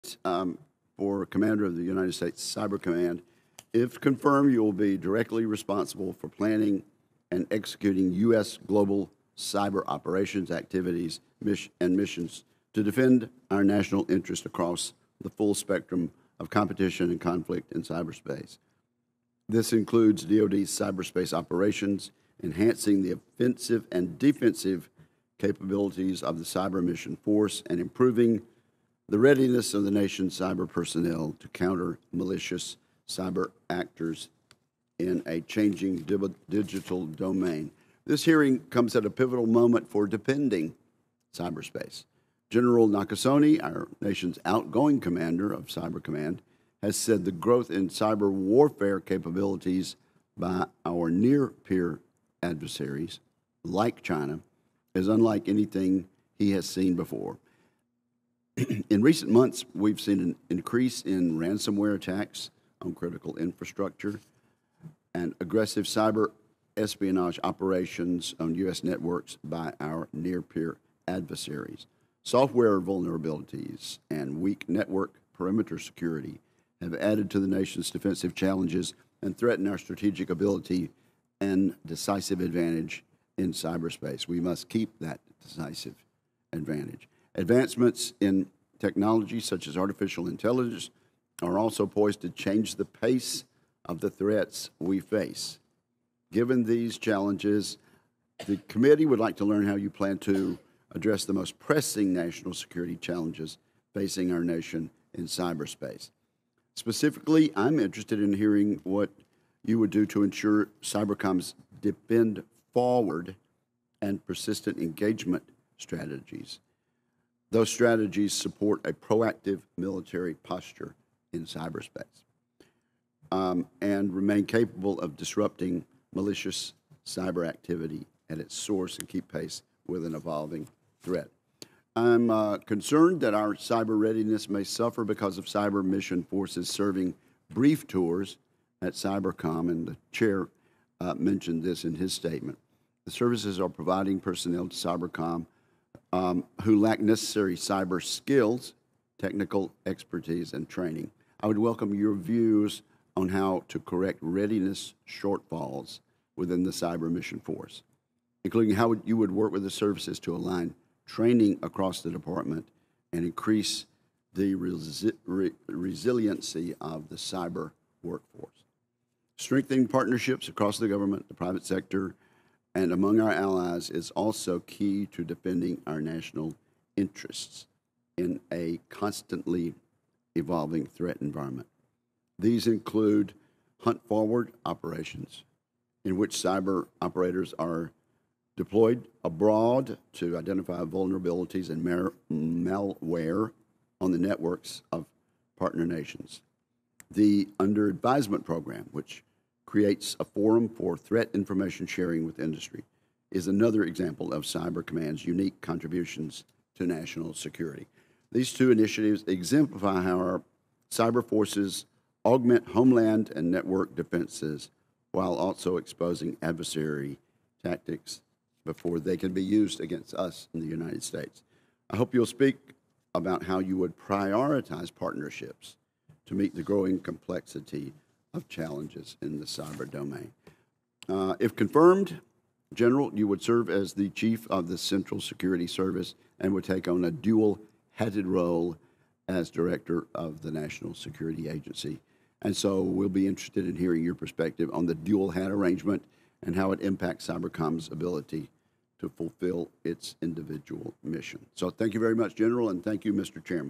Roger Wicker Warns Cyber Warfare Developments By China Are ‘Unlike Anything We’ve Seen Before’
On Thursday, Sen. Roger Wicker (R-MS) questioned Lieutenant General Haugh on cyber security strength during a Senate Armed Services Committee hearing.